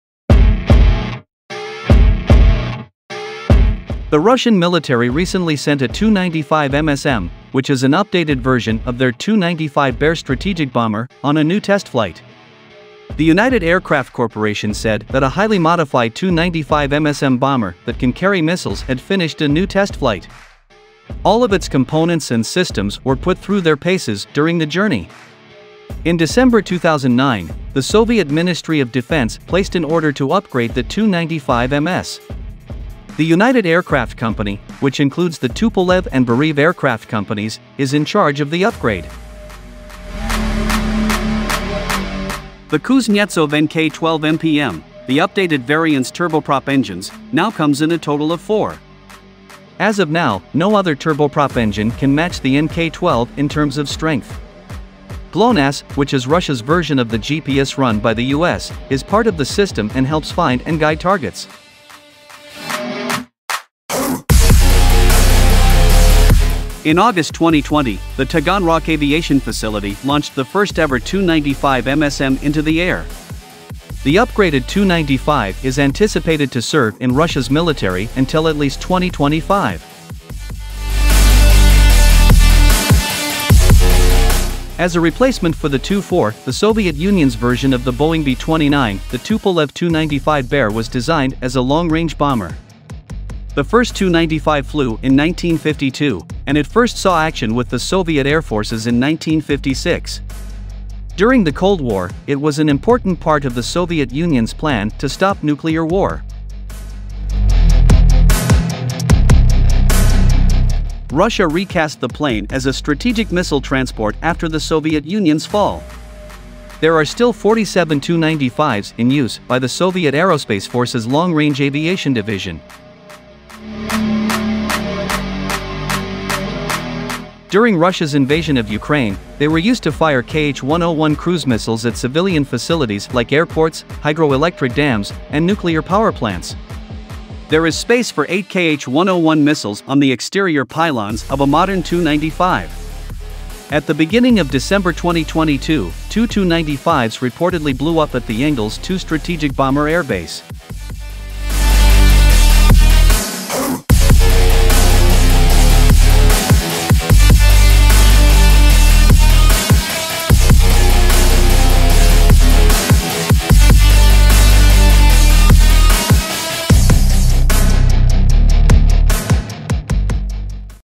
Russia's Tu-95MSM modern strategic bomber conducts a test flight after upgraded
The Russian military recently sent a Tu-95MSM, which is an updated version of their Tu-95 Bear strategic bomber, on a new test flight. The United Aircraft Corporation said that a highly modified Tu-95MSM bomber that can carry missiles had finished a new test flight. All of its components and systems were put through their paces during the journey. In December 2009, the Soviet Ministry of Defense placed an order to upgrade the Tu-95MS. The United Aircraft Company, which includes the Tupolev and Beriev Aircraft Companies, is in charge of the upgrade.